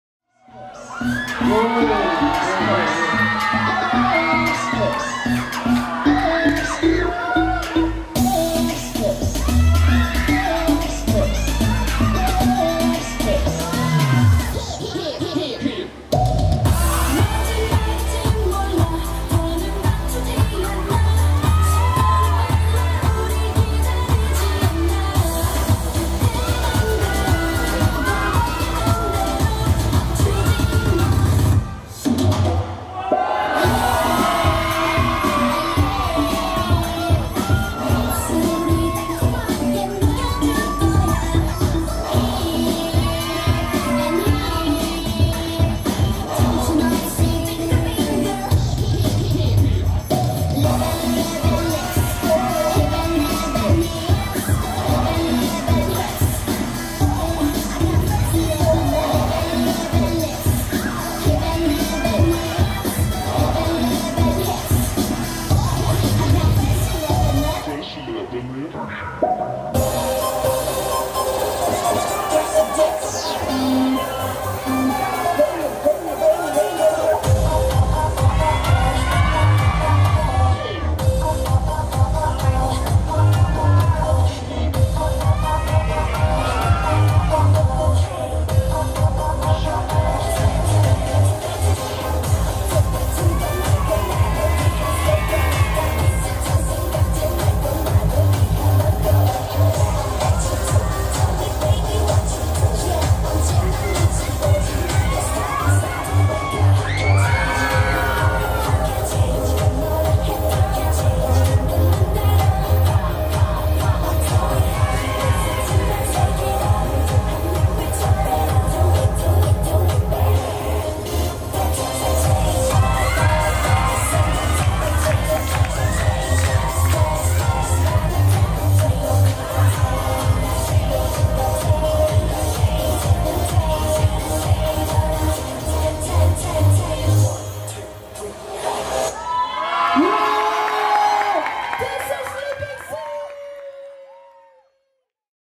Liping Sim | Judge Showcase | Aeon Mall Do Dance Carnival 2017
Liping Sim from Malaysia
Judge Showcase

➡️Judges
Michiyo Ho
Liping Sim (MALAYSIA)
Kidinn (MALAYSIA)
Bear Teo (MALAYSIA)
Boogaloo Hanz (MALAYSIA)
Brandon Tan (MALAYSIA)
Kai Lim (SINGAPORE)

➡️DJ
DJ William (MALAYSIA)